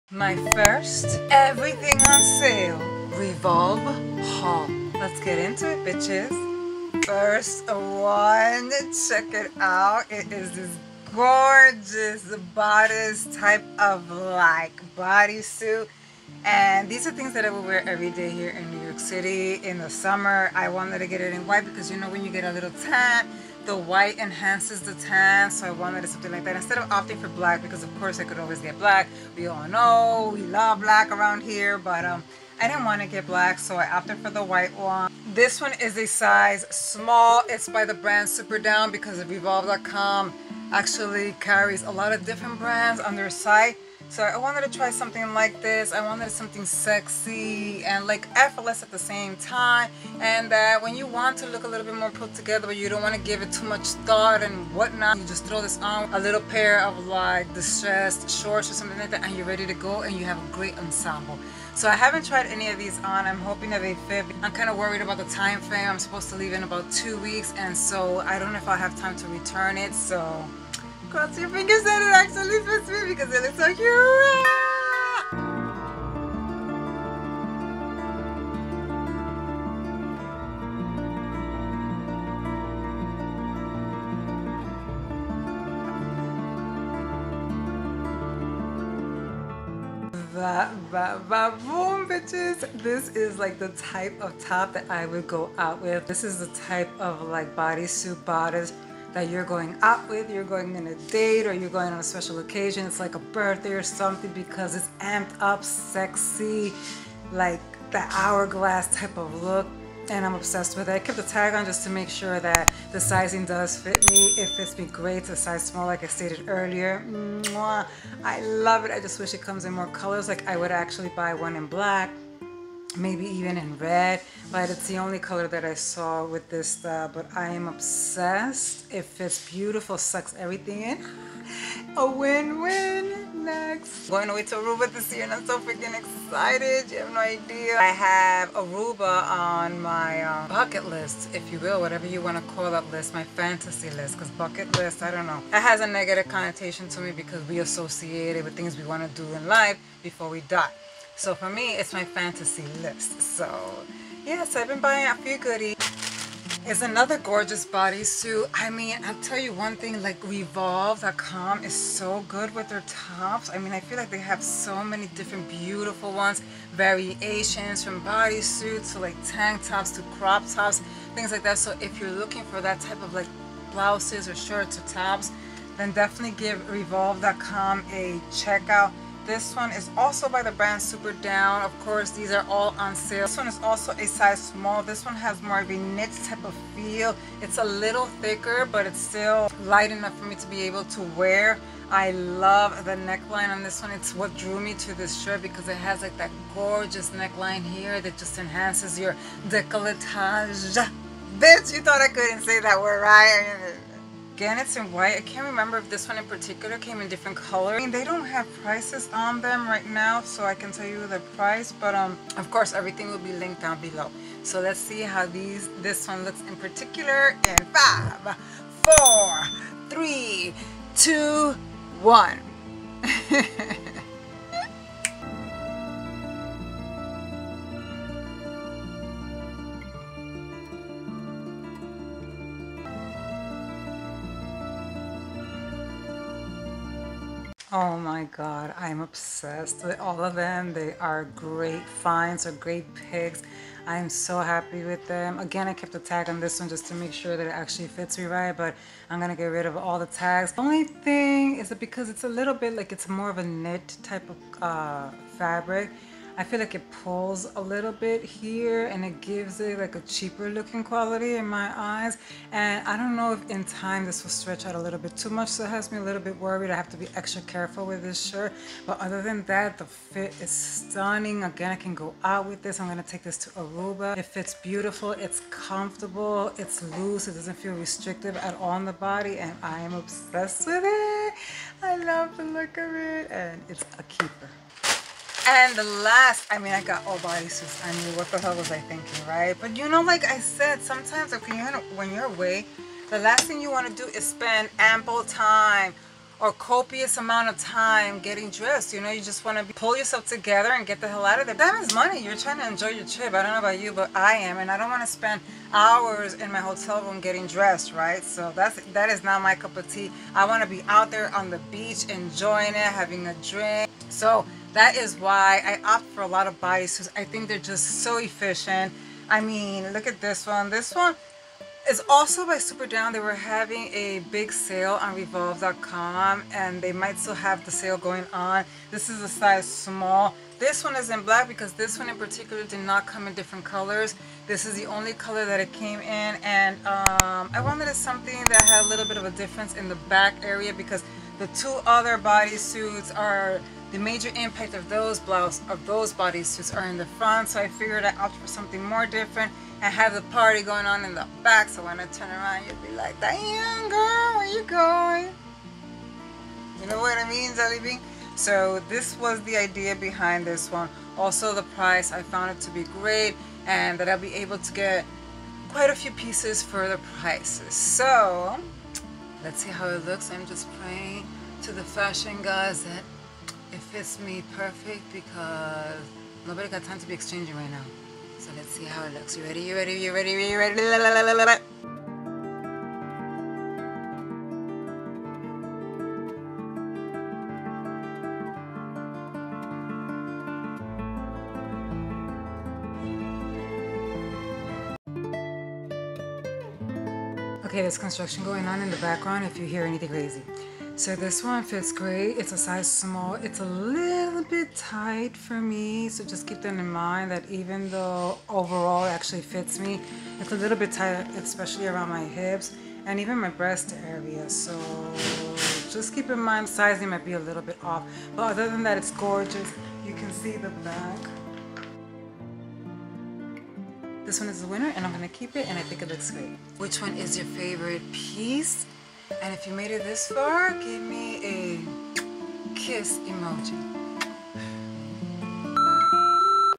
EVERYTHING ON SALE REVOLVE SUMMER 2022 HAUL: (BODYSUIT EDITION)
EVERYTHING ON SALE REVOLVE SUMMER HAUL 2022: (BODYSUIT EDITION) sexy My first Revolve Haul on my Channel. I went straight to the Sale Section and Hauled some of the sexiest BodySuits Revolve has to offer in this Summer Everything on Sale Revolve Summer 2022 Haul: BodySuit Edition.

UNBOXING THE NEW VERSACE MEDUSA AEVITAS PLATFORM PUMPS

BODYSUIT I'M WEARING: LOCAL STORE
EARRINGS: LOCAL STORE
 REVOLVE HAUL: